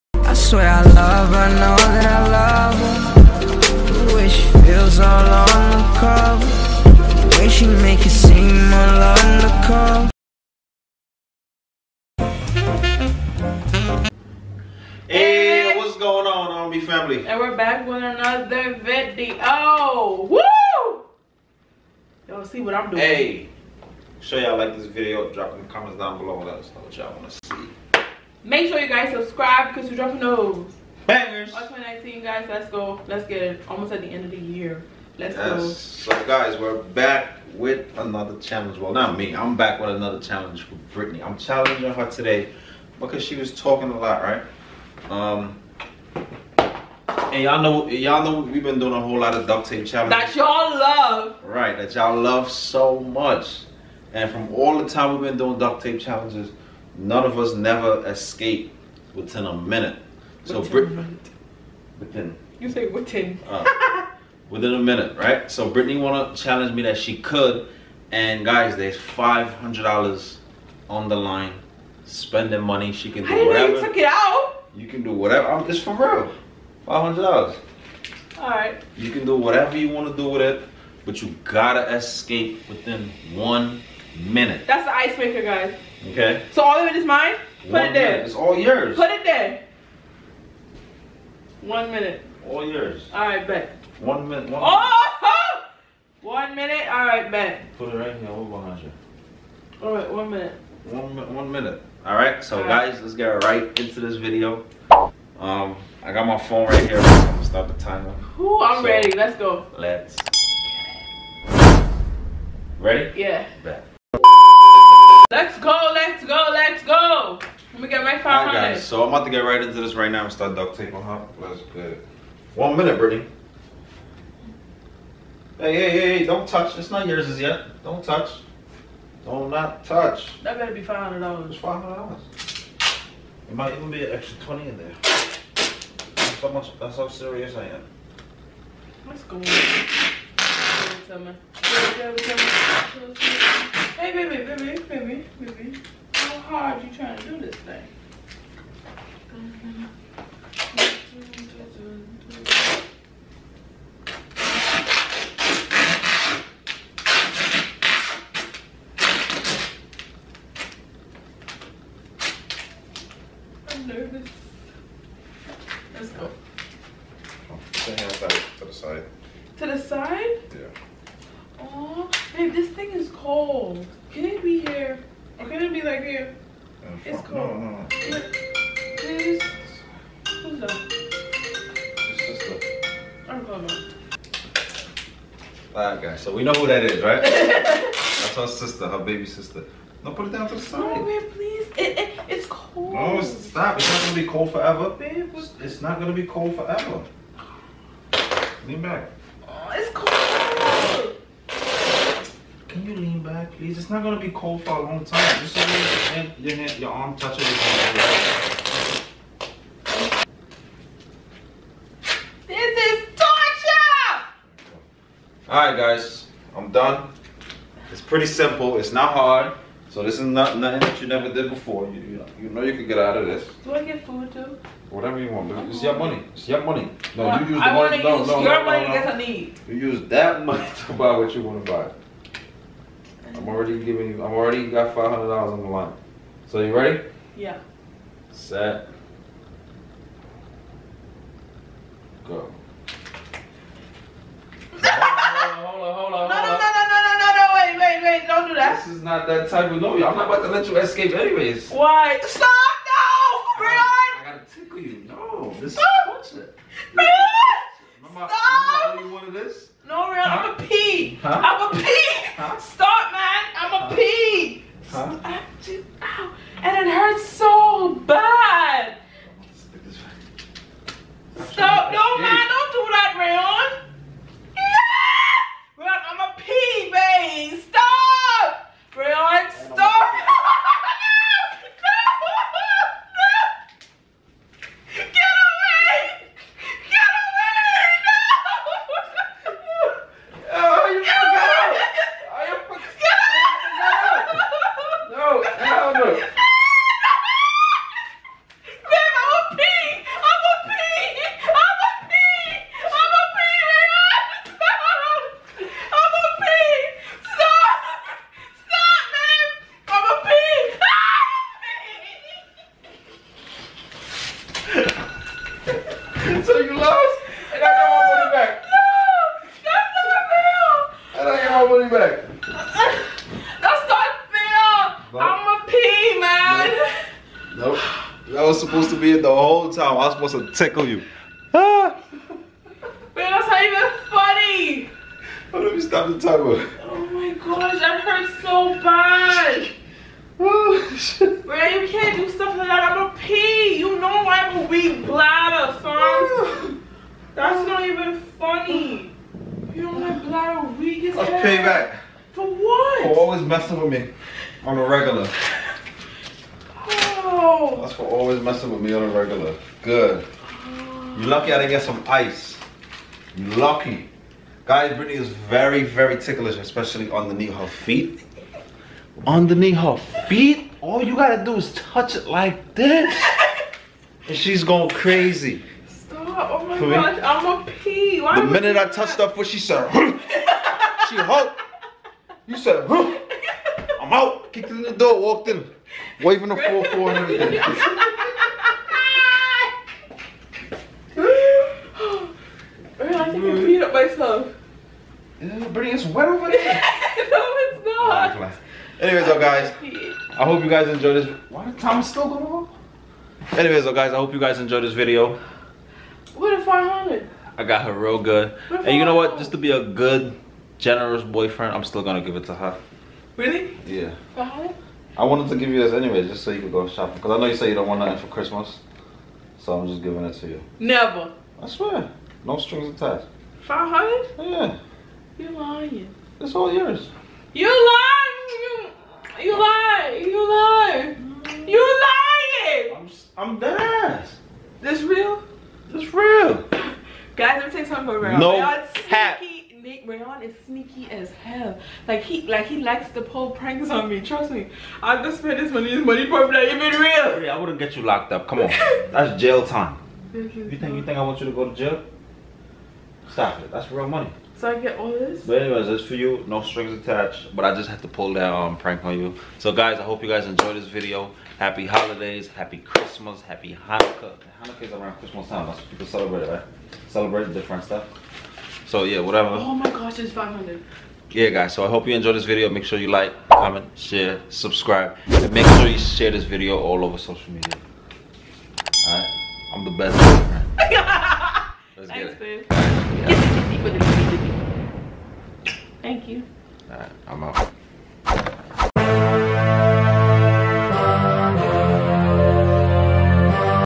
Extreme Duct Tape Tickle Challenge On Girlfriend!! *Peed herself* ($500 Cash)
Extreme Duct tape  tickle challenge on girlfriend!! *Peed herself* I told her if she can escape before 1 minute I will giver $500 Dollars Cash.

- PRODUCTS/VIDEO PROPS:
YOUTUBE PILLOW CASE
CAMERA USED
RING LIGHT
50inch TRIPOD
MINI TRIPOD